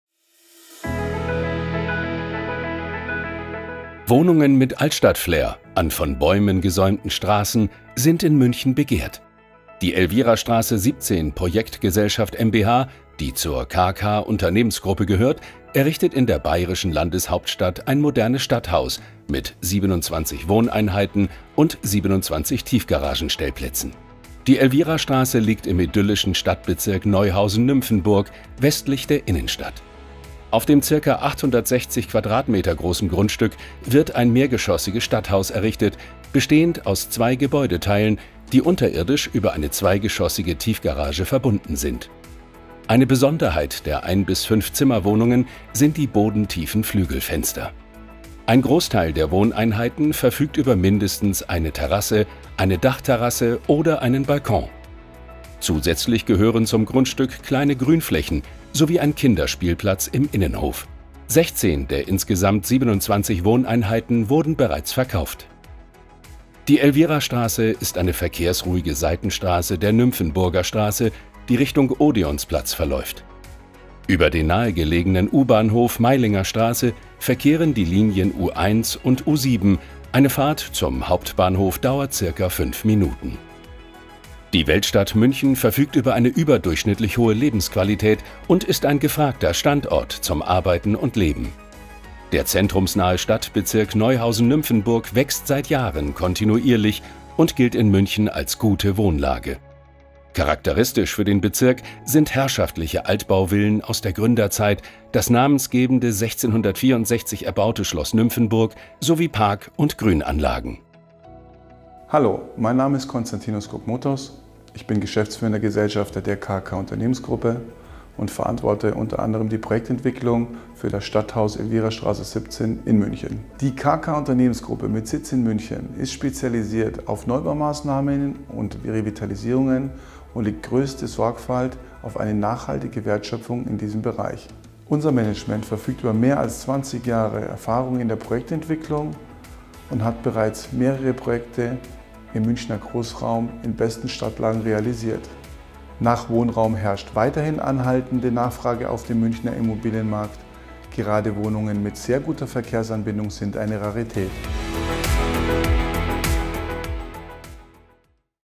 Engel & Völkers Digital Invest - Stadthaus "Mozart" IV
Im idyllischen Stadtbezirk Neuhausen-Nymphenburg, westlich der Innenstadt, errichtet die Elvirastr. 17 Projektgesellschaft mbH das Stadthaus “Mozart“, ein klassisch-elegantes Mehrfamilienhaus mit hochwertiger Ausstattung.

Die beiden mehrgeschossigen Gebäudeteile sind unterirdisch über eine zweigeschossige Tiefgarage verbunden. Das Stadthaus umfasst 27 Wohneinheiten sowie 27 Tiefgaragen- und 51-Fahrrad-Stellplätze.